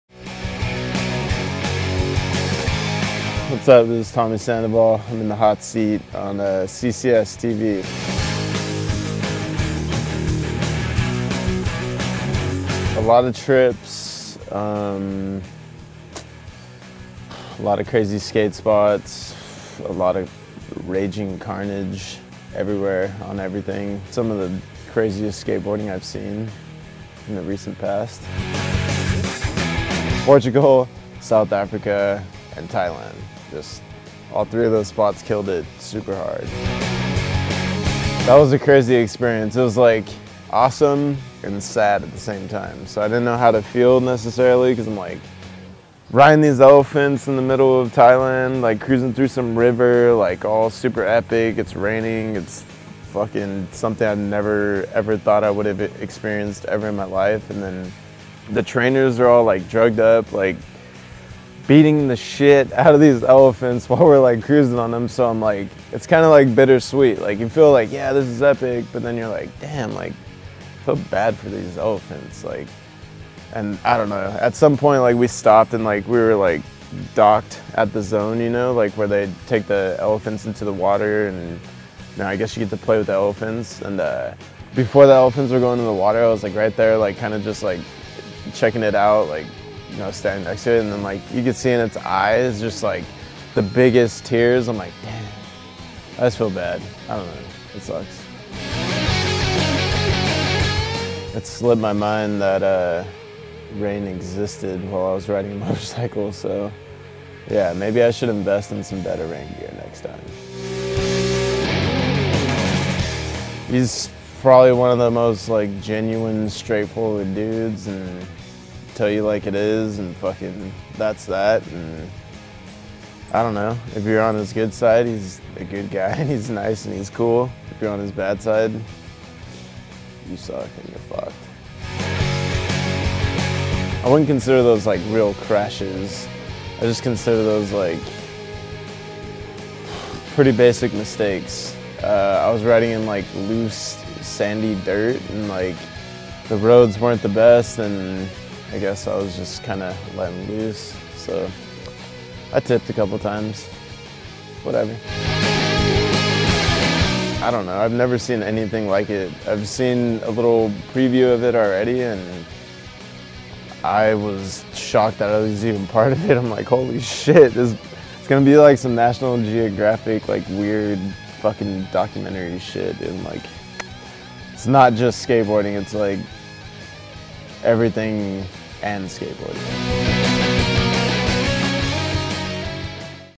The Hot Seat | Tommy Sandoval
In honor of the upcoming release of Fallen's long anticipated new video, Road Less Traveled, CCS cornered Fallen Pro Tommy Sandoval and sat him down in The Hot Seat for a little grill session to find out everything that went into it. Tommy talks about some his favorite roads traveled, his adventures with Elephants in Thailand and give an overall breakdown of the caliber of skateboarding we're to expect from Road Less Traveled. All this and more in this episode of The Hot Seat.